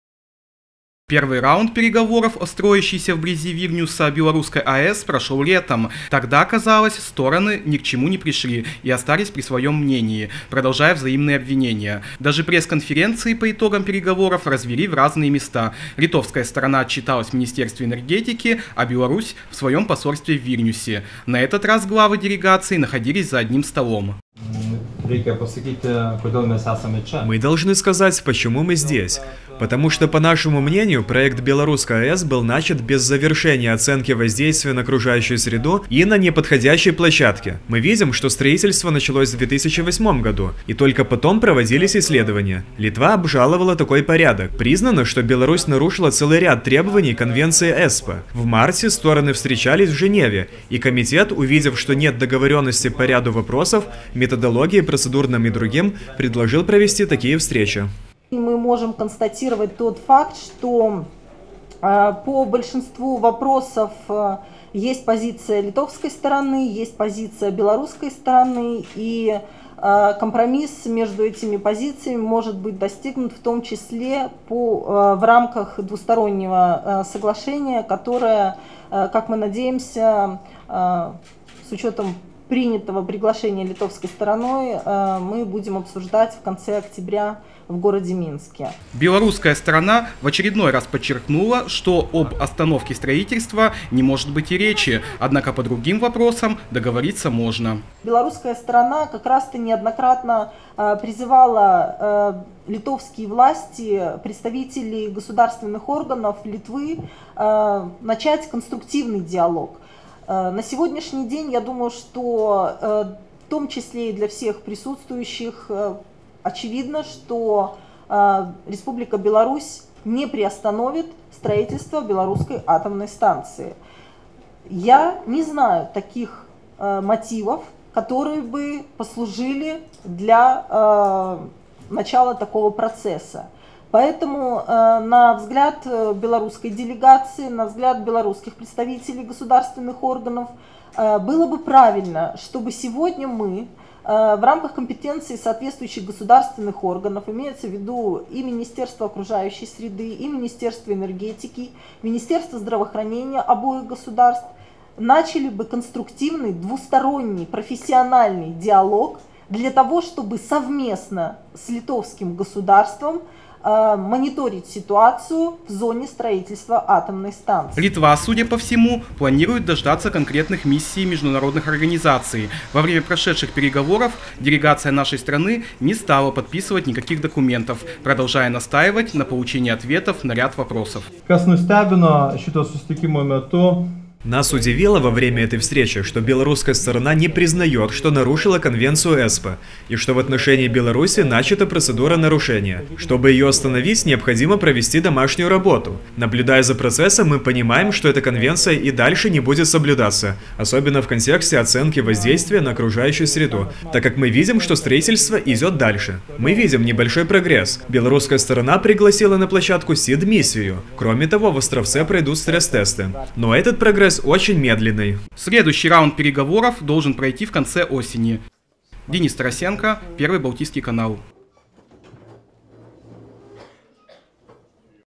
БелАЭС: трудный путь переговоров
А в Минске завершился второй раунд литовско-белорусских переговоров о БелАЭС, которую строят всего в 50 км от Вильнюса. Стороны продолжают отстаивать свои, часто противоположные, позиции и со скрипом движутся к компромиссу, которого пока не видно.